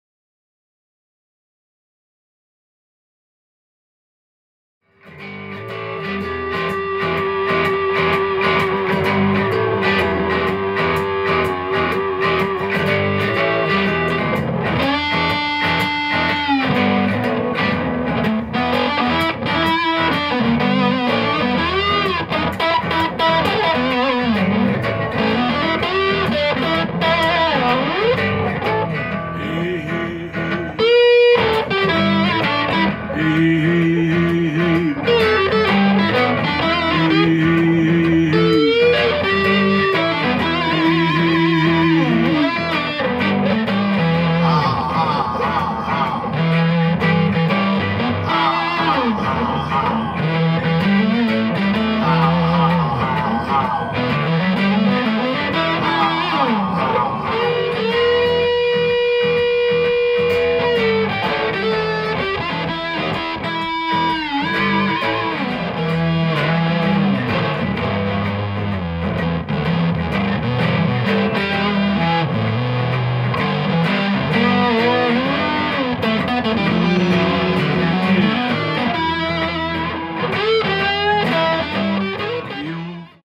Big Muff Pi Ram's Head V2 Demo
Here's a quick and dirty demo of my Big Muff Pi (Ram's Head V2). Played through a Quilter Aviator Cub, set pretty clean, with a bit of delay and tremolo.